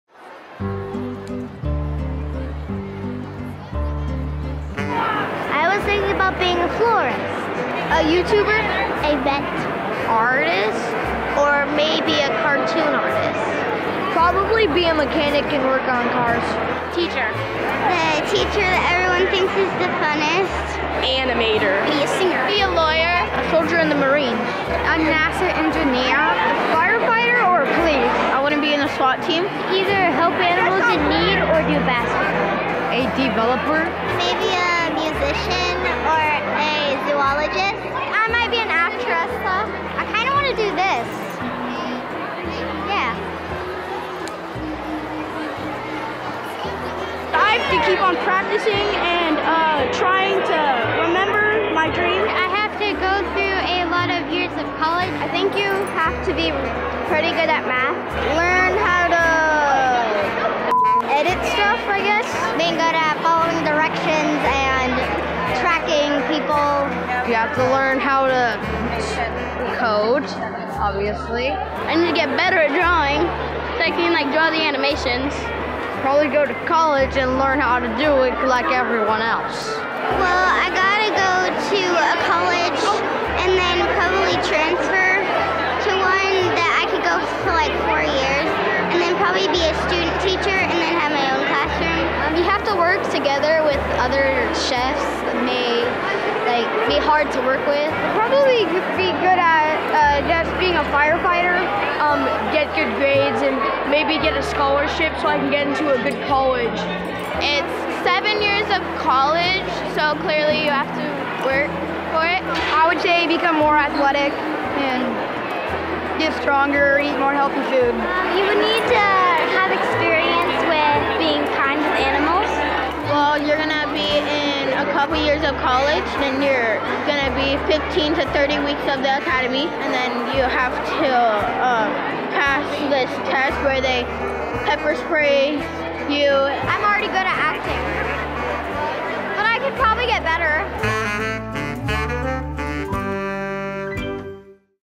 5th graders: What's your dream job & how will you get it?
On Nov. 7, 2017, our 5th Grade Career Fair brought together 2,600 students, 220 career professionals, and 39 elementary school counselors.
Learn more about our district's goal to prepare students for high school and beyond,